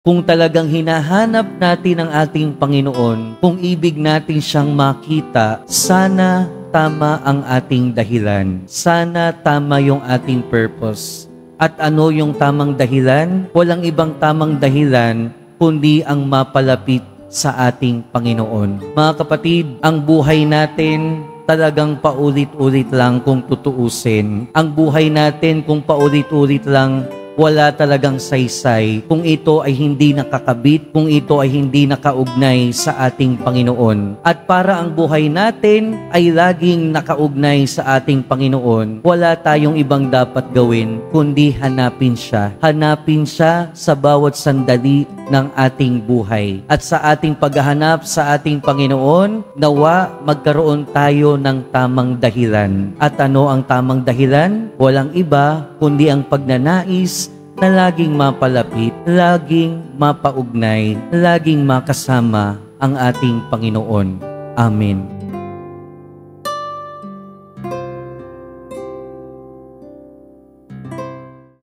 Salya sa Biyaya
09-26-24 | SALYA sa BIYAYA: "Ang buhay natin talagang paulit-ulit lang kung tutuusin, ang buhay natin kung paulit-ulit lang wala talagang saysay kung ito ay hindi nakakabit, kung ito ay hindi nakaka-ugnay sa ating Panginoon."- Rev. Fr. Christopher S. Crucero, LRMS (Parochial Vicar)